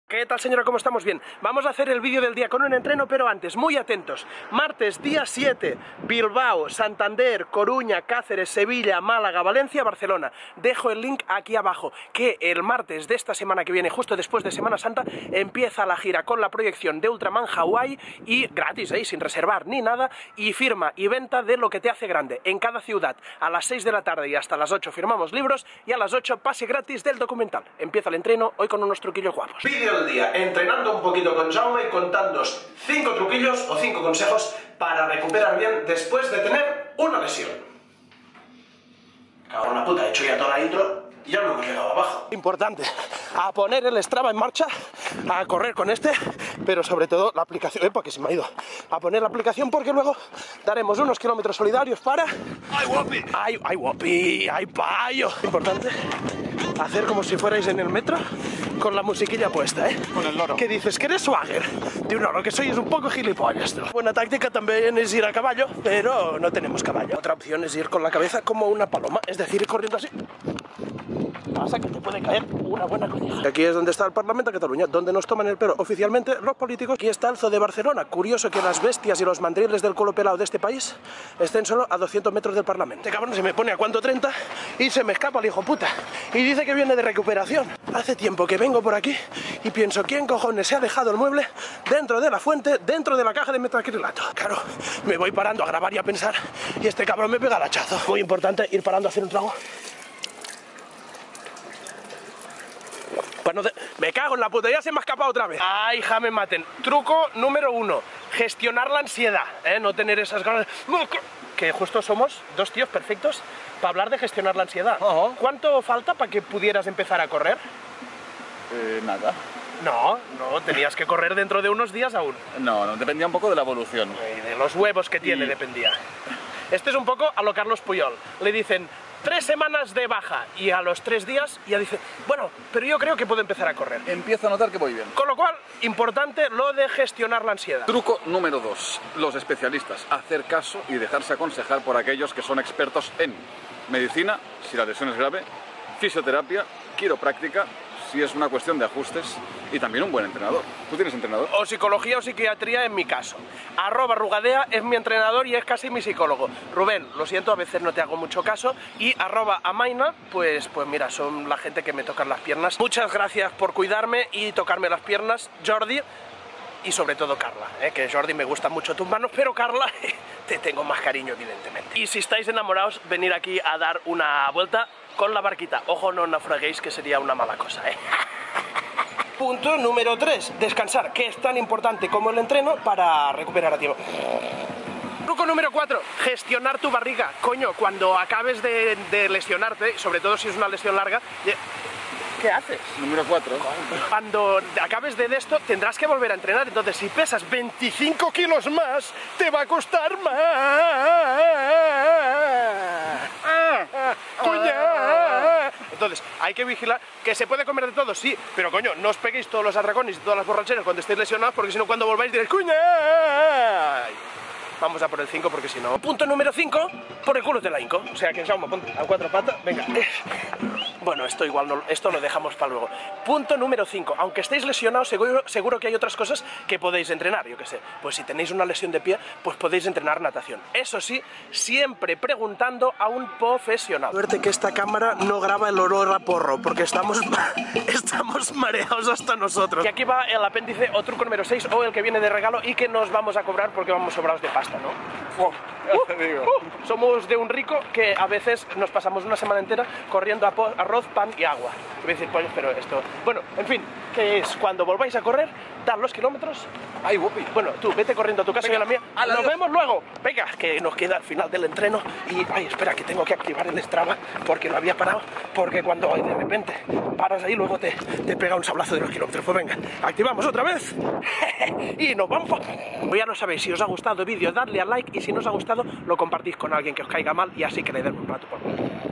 5 Trucos Para Recuperar Lesiones
Aquí van 5 truquillos para recuperar mejor una lesión con @jaumegoma mientras hacemos el tercer entreno del día!